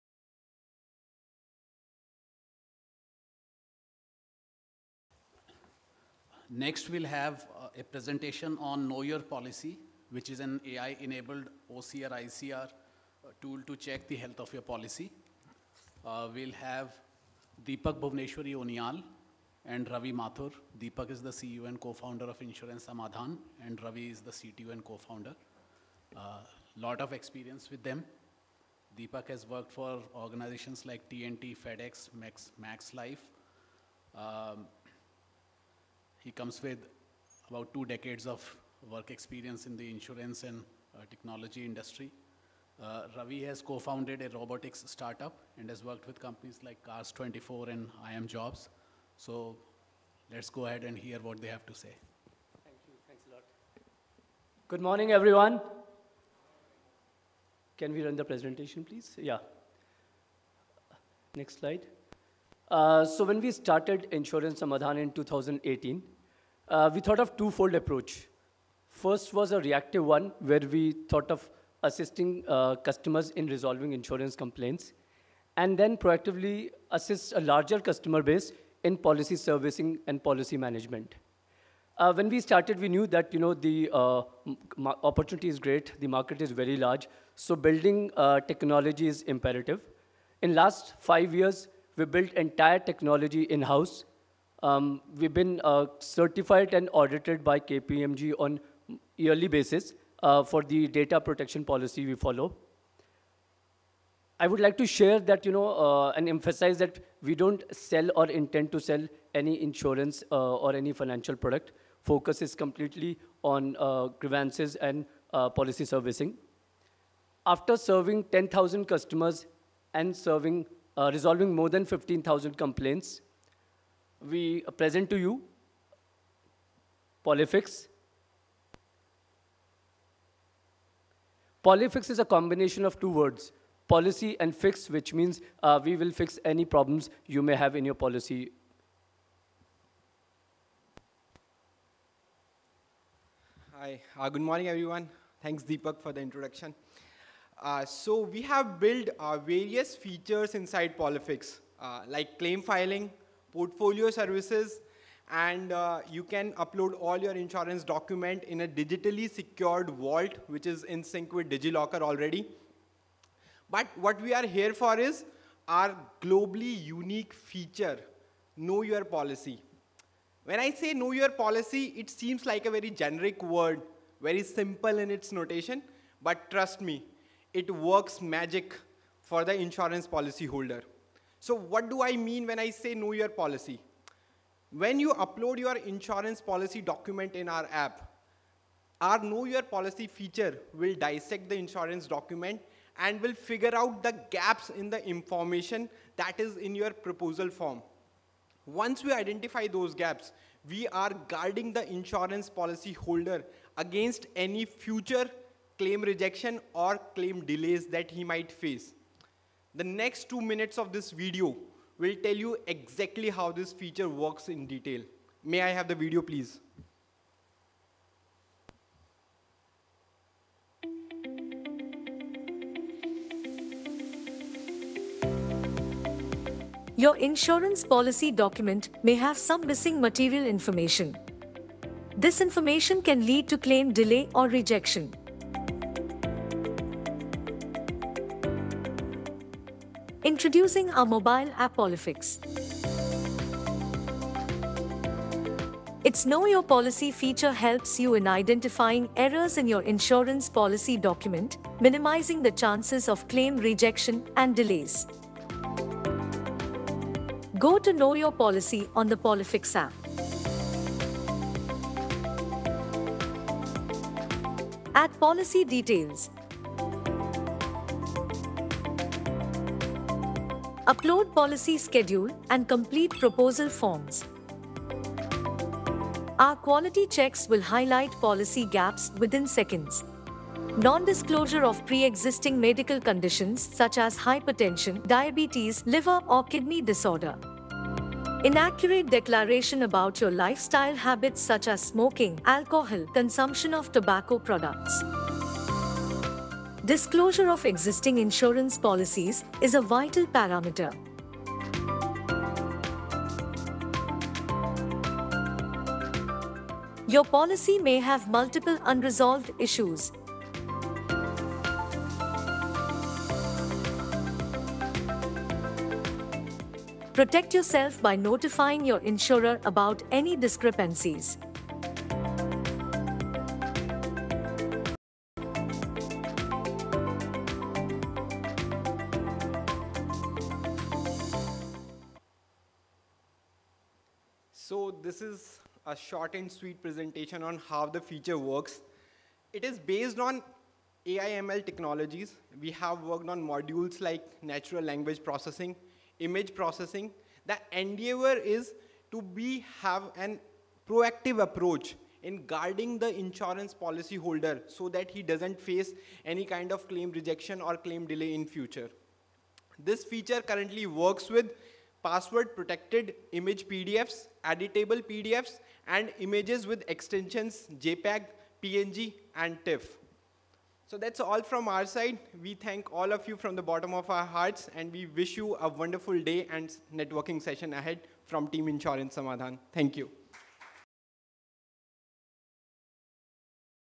IIA Annual Event | Introducing KYP - AI enabled OCR/ICR tool to check health of your policy
Check out the insightful demo by Mr. Deepak Bhuvneshwari Uniyal, CEO & Co-Founder, Insurance Samadhan, and Mr. Ravi Mathur, CTO & Co-Founder, Insurance Samadhan, on the topic of "Introducing KYP (Know your policy) - AI enabled OCR/ICR tool to check health of your policy" at the India InsurTech Annual Event 2023!